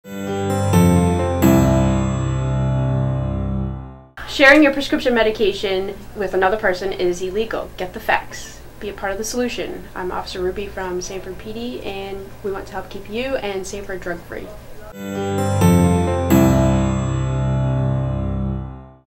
Officer Ruby from Sanford PD and Y
Healthy awareness and prevention messages from friends and staff of Y of Sanford-Springvale, focusing on RX abuse prevention for teens.